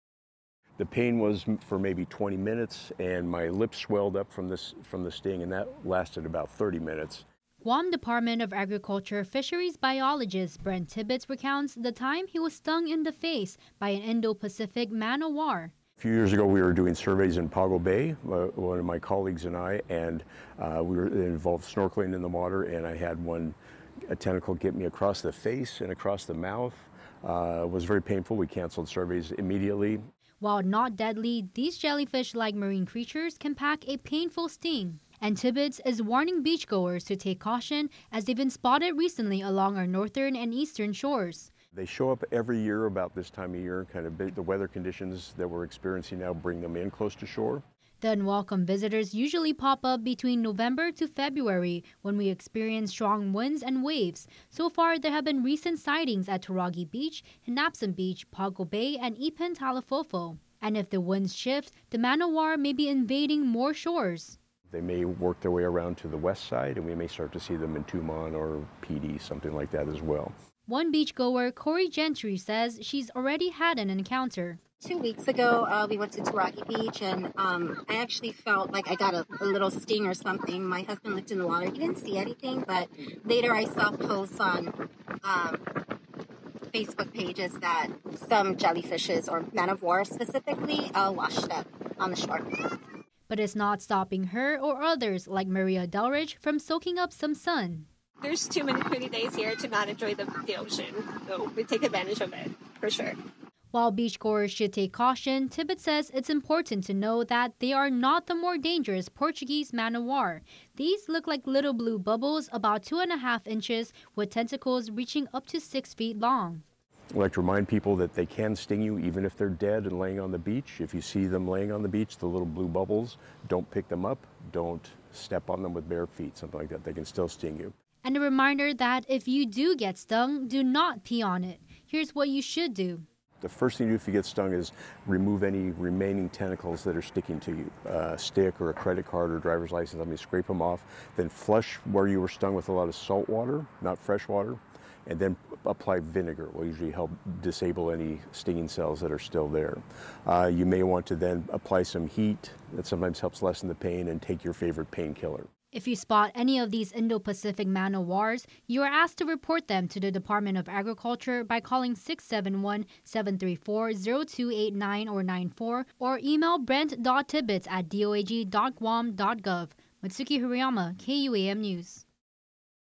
Indo-Pacific Man o' War spotted in north and east facing beaches
A warning to beachgoers this holiday weekend. If you see bubble-like creatures washed up on shore, take caution. The Indo-Pacific Man o’ War has been spotted at several northern and eastern beaches from Ritidian to Talofofo.